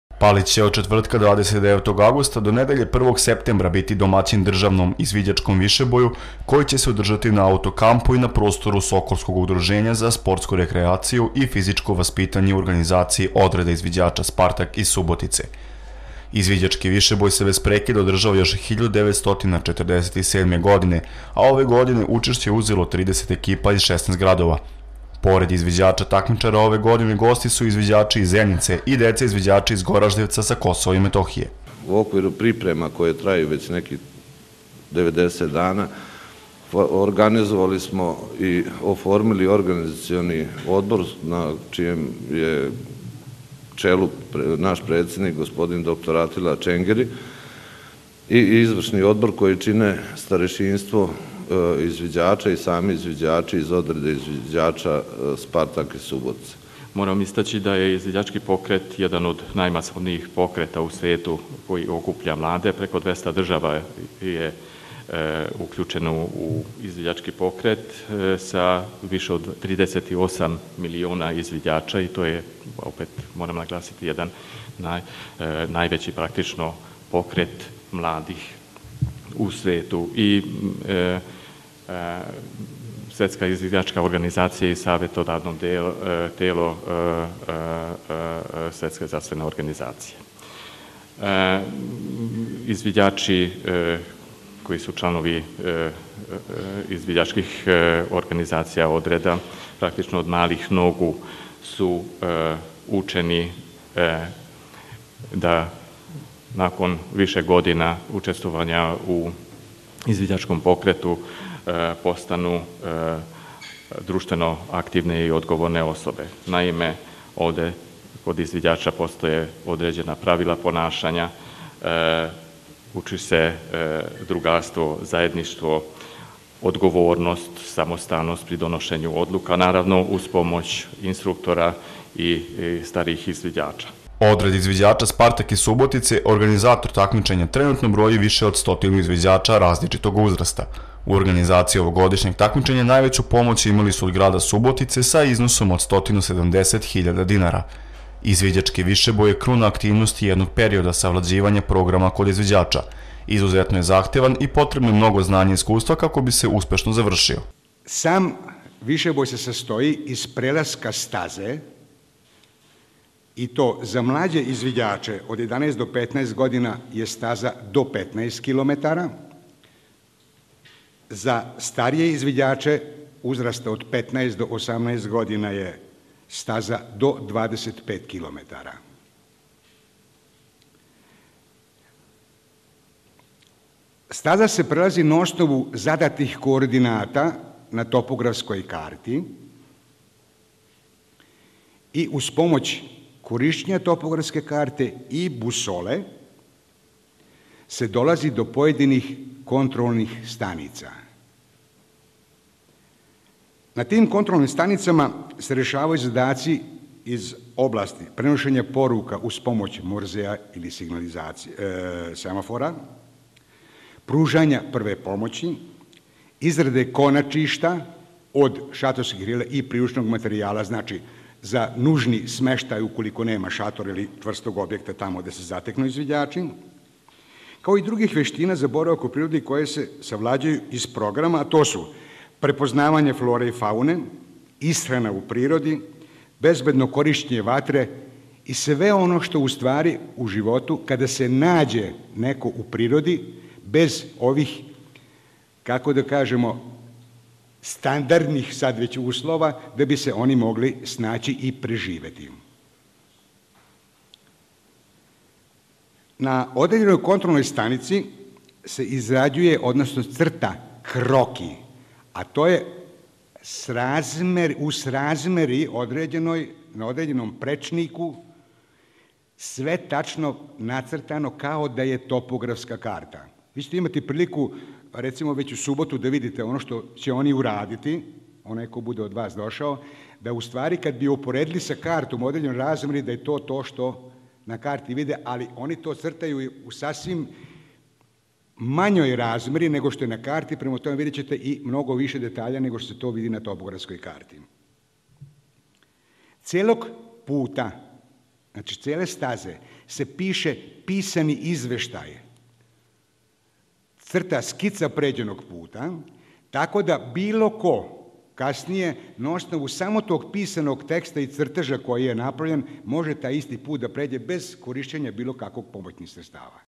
Palić je domaćin državnom izviđačkom višeboju
Palić će od četvrtka 29. avgusta do nedelje 1. septembra biti domaćin državno  izviđačkom višeboju koji će se održati na autokampu i na prostoru Sokolskog udruženja za sportsku rekreaciju i fizičko vaspitanje u organizaciji odreda izviđača Spartak iz Subotice.

Izviđački višeboj se bez prekida održava još od 1947. godine, a ove godine učešće je uzelo 30. ekipa iz 16. gradova. Pored izviđača takmičara ove godine gosti su izviđači iz Zenice i deca izviđači iz Goraždevca sa Kosova i Metohije. Odred izviđača Spartak iz Subotice organizator takmičenja trenutno broji više od 100. izviđača različitog uzrasta. U organizaciji ovogodišnjeg takmičenja, najveću pomoć imali su od grada Subotice sa iznosom od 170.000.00 rsd. Izviđački višeboj je kruna aktivnosti jednog perioda savlađivanja programa kod izviđača. Izuzetno je zahtevan, i potrebno je mnogo znanja i iskustva kako bi se uspešno završio.